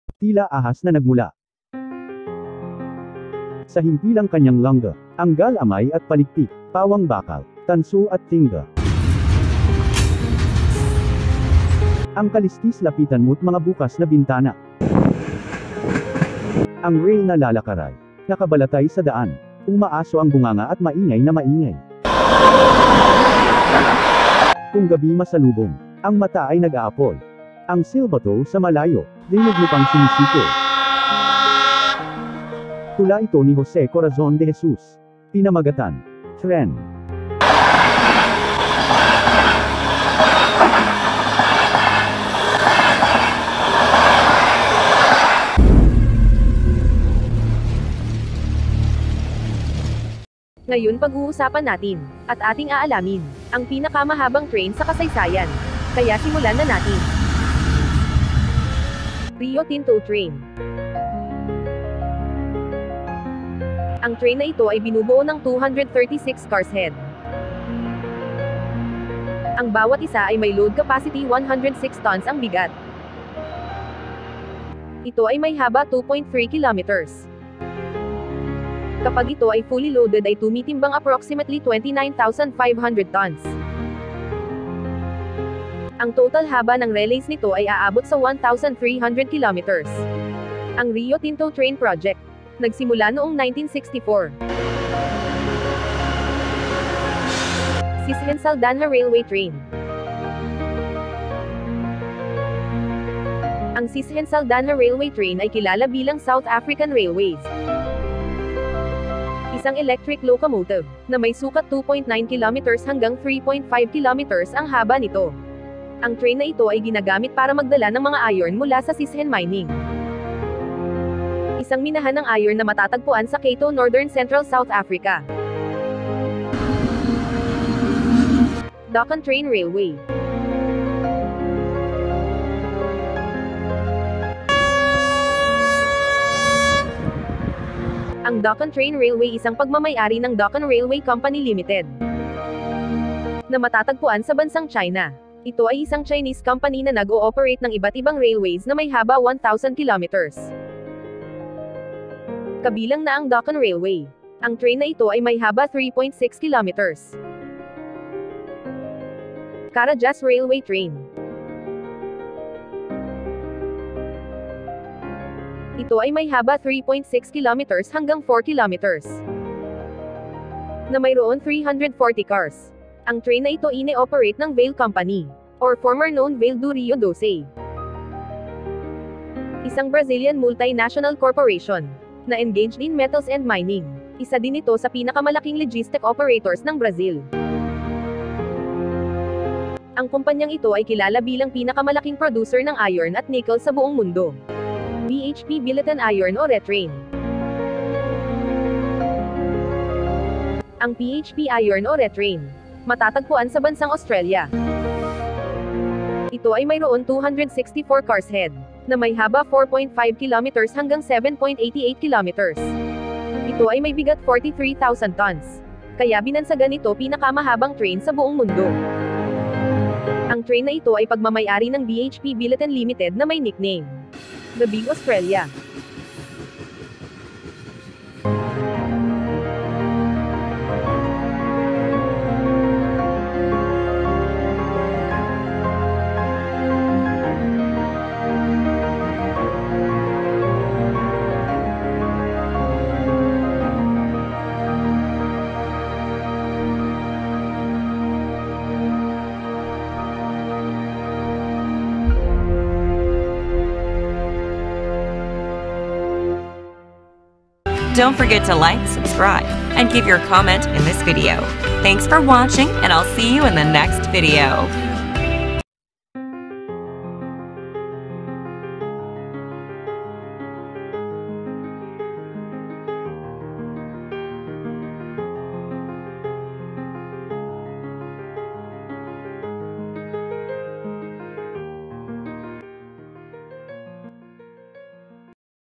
PINAKAMAHABANG TRAIN SA KASAYSAYAN |KNT-096
Narito ang 5 Pinakamahabang Train Sa Kasaysayan
1. BHP Billiton Iron Ore Train ( Australia)
2. Carajas Train Railway (Brazil)
3. Daqin Train Railway (China)
4. Sishen Saldanha Railway Train ( South Africa)
5. Rio Tinto Train (Australia)
FREE Training para sa mga Gusto maging Youtuber at matutunan kumita sa Youtube